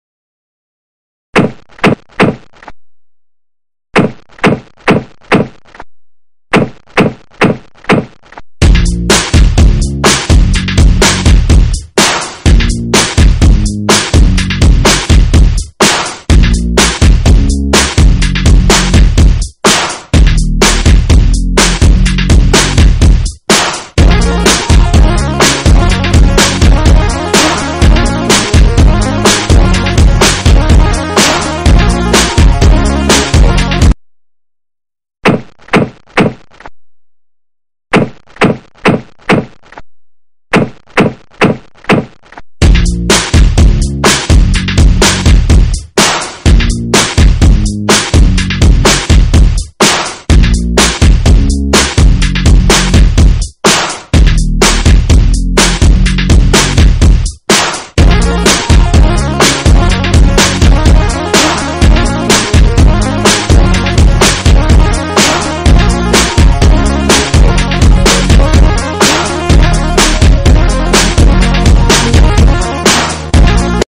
Kiddies Sport Montage
It's sport programe that designed to get the talented children that have a lot to offer their fatherland